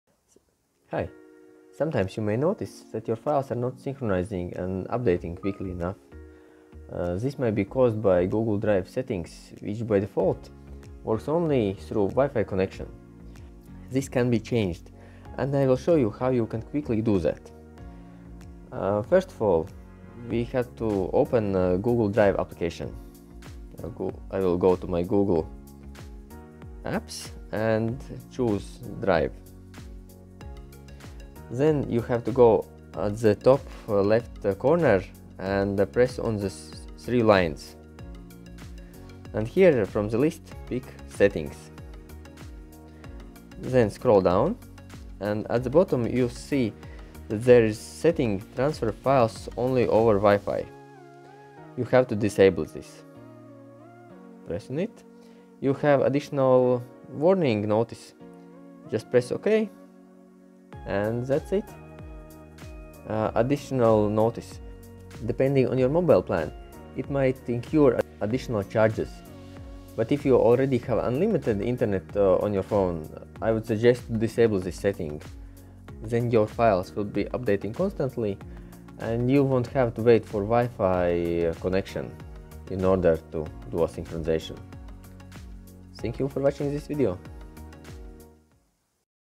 Google Drive on mobile phone not synchronizing without wi-fi. There is a quick fix.
Sometimes files are not synchronizing quickly enough to Google Drive on mobile phone. This can be because sync is enabled only through Wi-Fi. Mobile data is not used for this purpose. It is possible to enable also synchronization through mobile data. Watch this short video and you will see the fix.
This video was made on Android device - Samsung S10e.